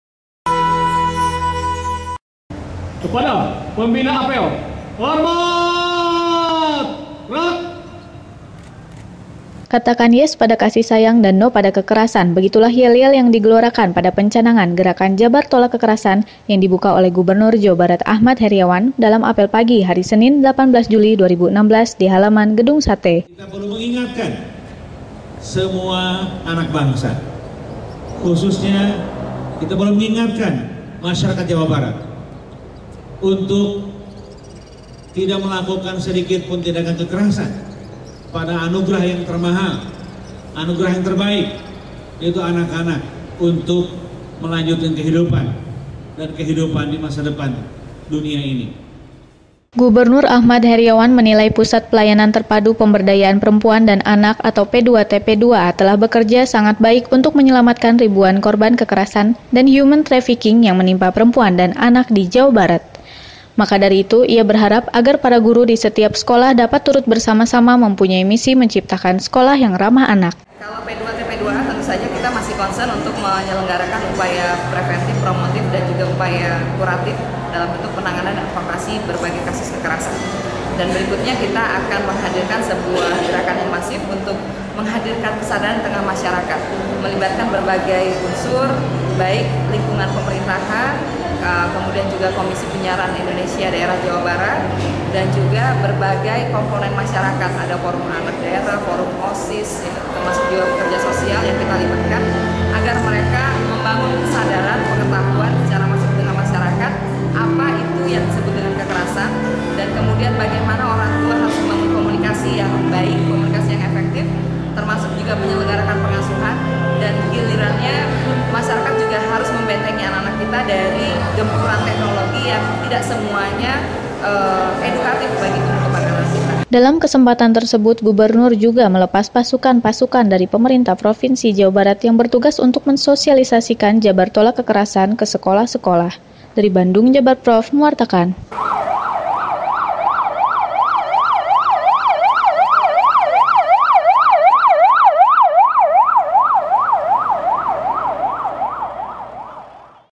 Jabarprov TV - Apel Besar Pencanangan Gerakan Jabar Tolak Kekerasan
Gubernur Jawa Barat menjadi pembina apel pagi "Jawa Barat Tolak Kekerasan" pada hari Senin 18 Juli 2016 di Gedung Sate.